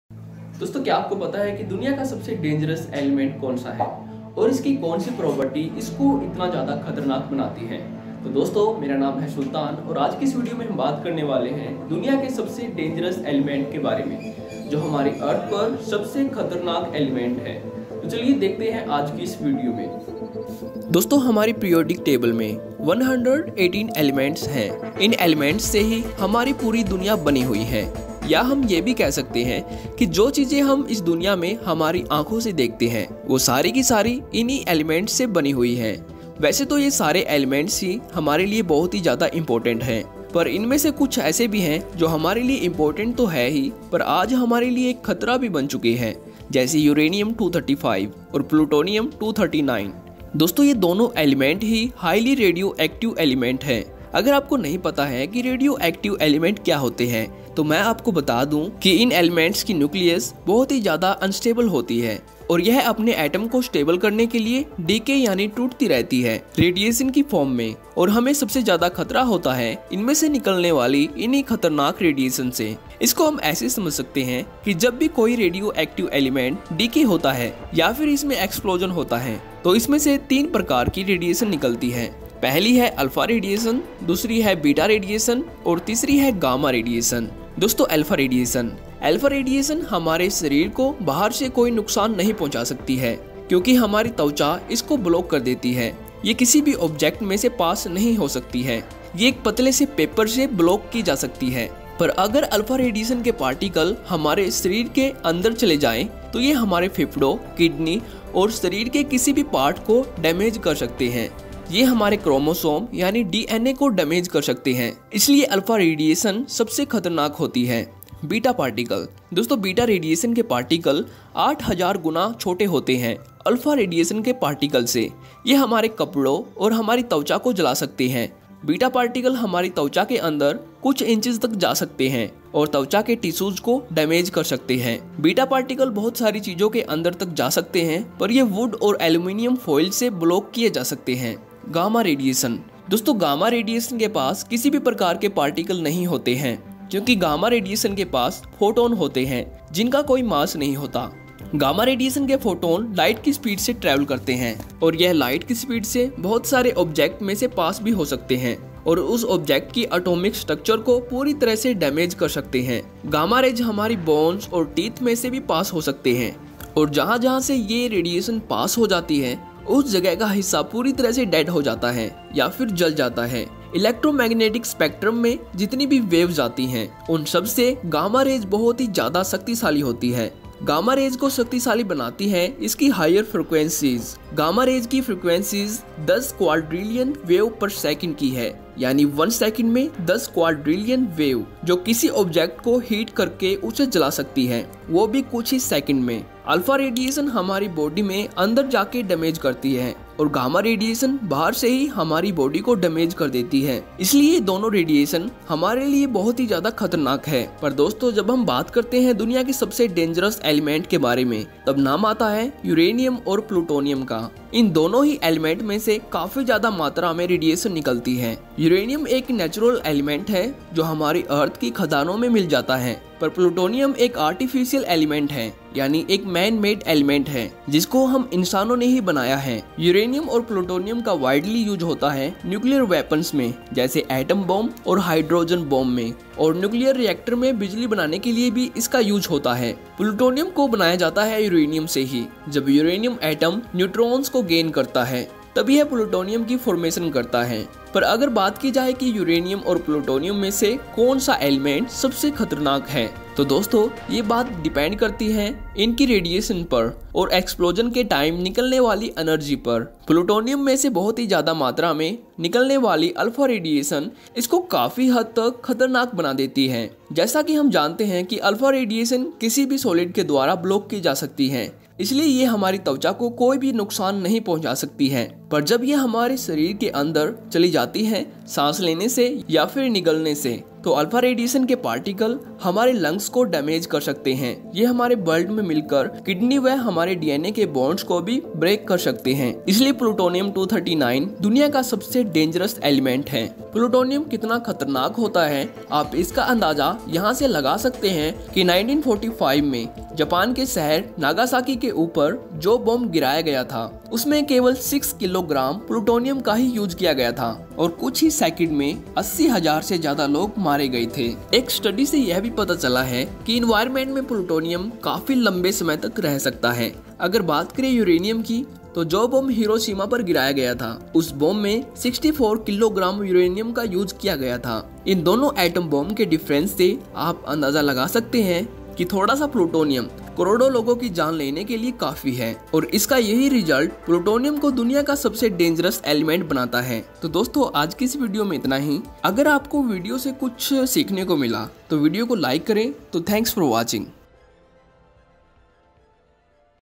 दुनिया का सबसे विषैला और खतरनाक तत्व || the world's most toxic and dangerous element
dosto kya apko pta hai ki duniya ka sabse dangerous elements konsa hai aur isko konsi chize itna dangerous bnati hai, aaj ki is video me ham bat krne wale hai world's most dangerous elements k bare me. uranium 235 and plutonium 239 ko ham details se smjenge.